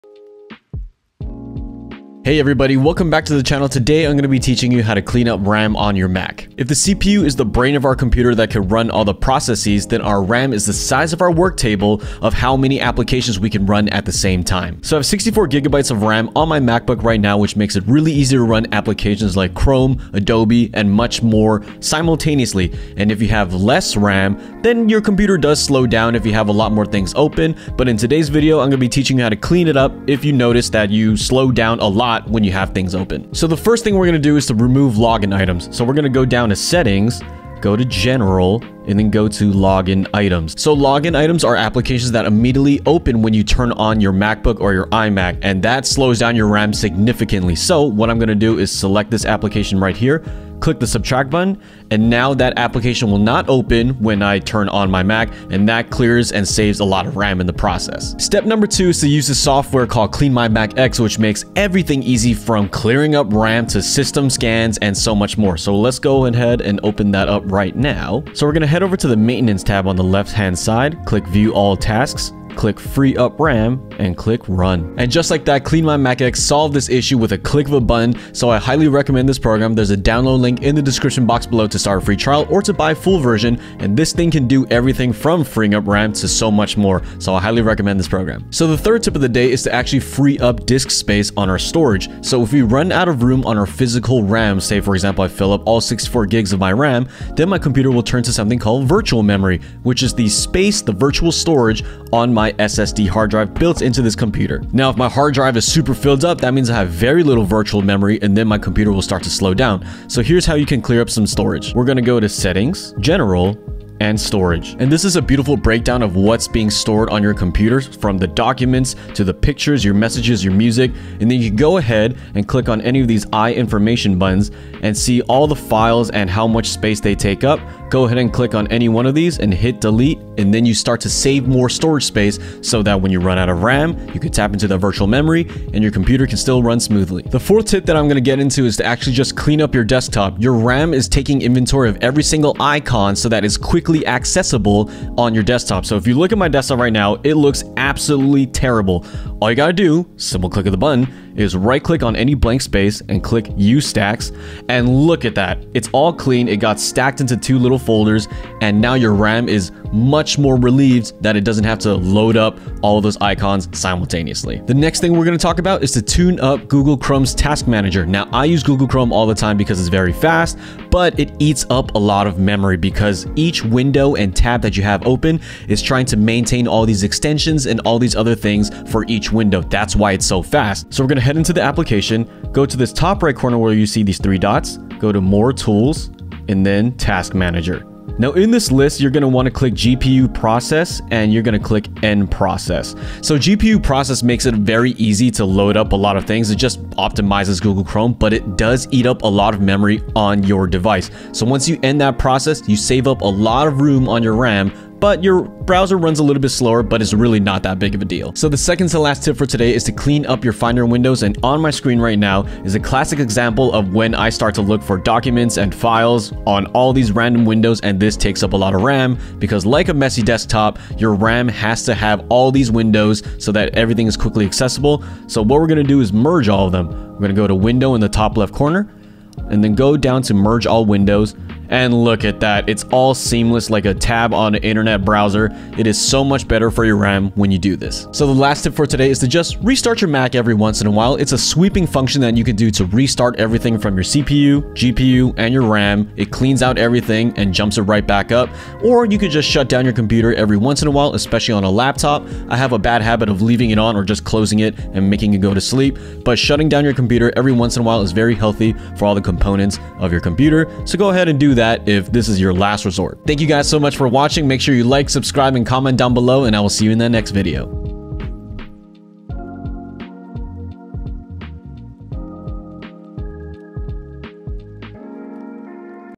7 Tricks to Free Up Mac’s RAM in No Time
What To Do If Your RAM on Mac Is Cluttered?
Try CleanMyMac X for free

Sluggish Mac? There is a solution! Clean up your RAM to instantly optimize and speed up your Mac.

RAM, random access memory, is where apps, data, and components of the operating system in use are stored.

How to check RAM usage?
- Open Activity Monitor.
- Look at Memory Pressure. If your chart is primarily red and yellow, your Mac cries for fresh memory.

How to reduce Mac’s memory usage:

1. Remove Login Items
System Settings - General. Look for Open at Login—select programs you don’t want to load on a startup and click the “–.”

2. Wipe out RAM in one click with CleanMyMac X:
Open the app and go to the Maintenance tab.
Click Free Up RAM, then click Run.

3. Free up disk space
Go to Settings - General - Storage. Here you can see the breakdown of your Mac storage. Choose the files you want to remove and click Delete.

4. Clean up your Desktop
Control-click your Desktop and select Use Stacks to keep your Desktop clean.

5. Tune up Chrome’s Task Manager
Chrome settings - More tools - Task Manager. Close the GPU process by clicking End Process next to it to free up RAM on your Mac.

6. Close Finder windows
Click on Finder - Window - Merge All Windows.

7. Restart your Mac

Stay RAM-free!

Contents:
00:00 - Intro
00:06 - What is RAM on Mac
00:33 - Tip 1: Remove login items
00:58 - Tip 2: Use CleanMyMac X
01:28 - Tip 3: Free up disk space
02:17 - Tip 4: Clean up your desktop
02:44 - Tip 5: Tune up Chrome Task Manager
03:28 - Tip 6: Merge Finder windows
03:58 - Tip 7: Restart your Mac
04:23 - Outro